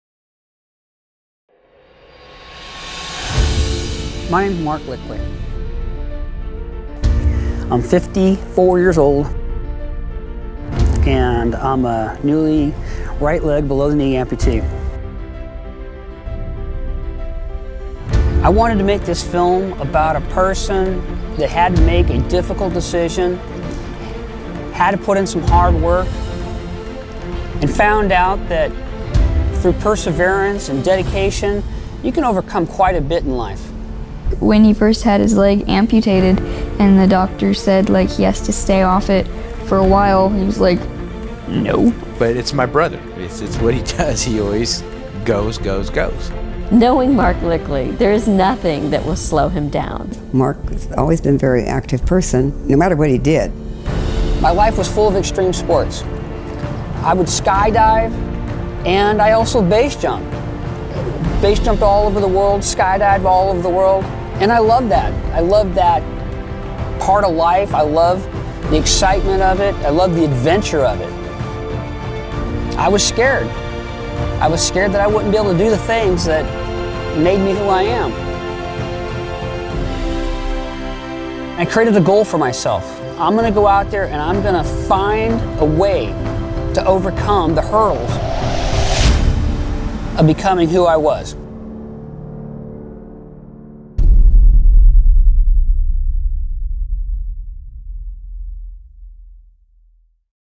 Inspire Trailer AE
This is just one of several sequences that I've produced about my life as an amputation. In the end I plan to produce an inspirational documentary designed to inspire others to overcome their hurdles.